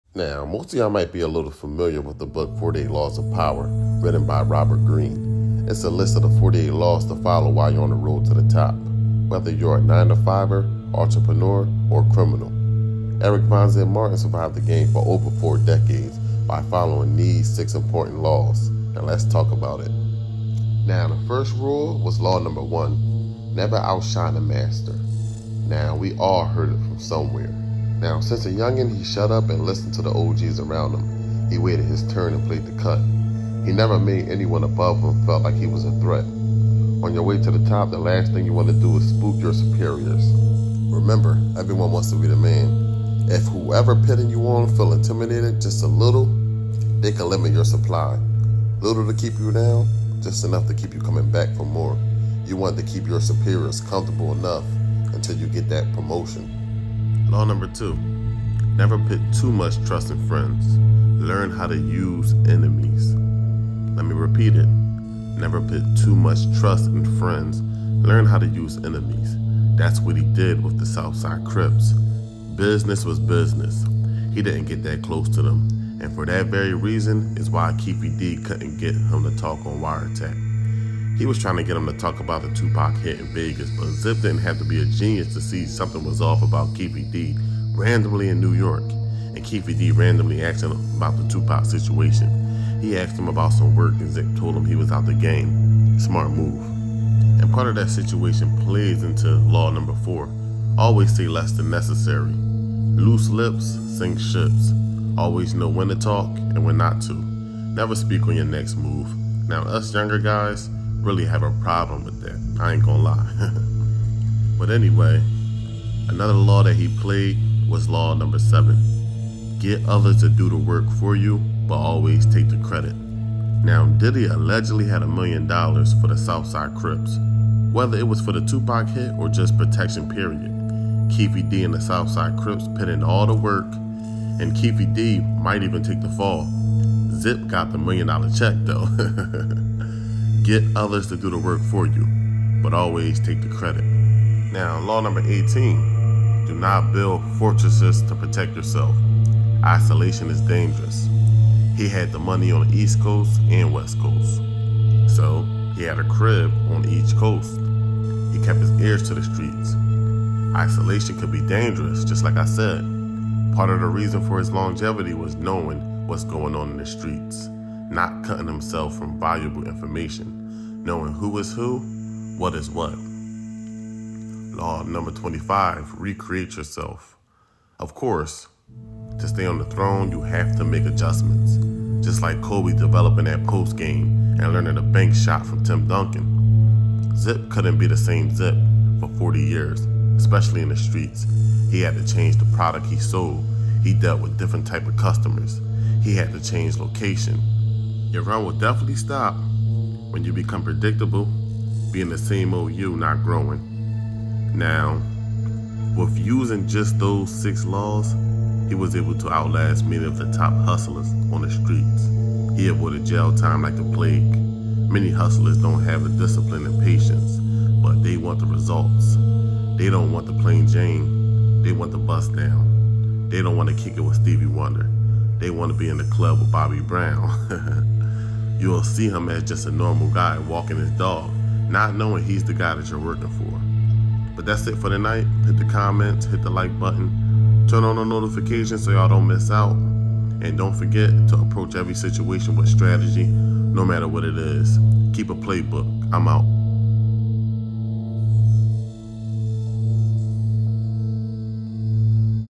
Laws of Power - Eric Von Zip Martin
With all the noise behind the Tupac and Keefe D situation, Eric Von Zip name seems to be in the whispers. His alleged involvement with connecting Diddy with the South Side Crips